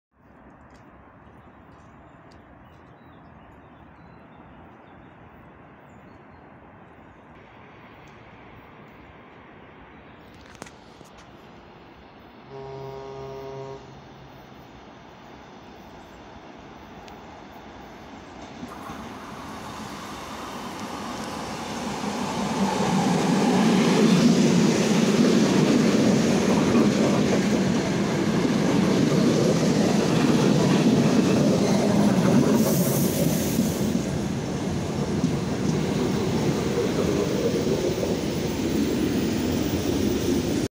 "Новый рейс" воздуховоз Эр9м-550 №7006 Буча-Святошин пл.Новобеличи перегон Беличи-Святошин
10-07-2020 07:36 -2 от графика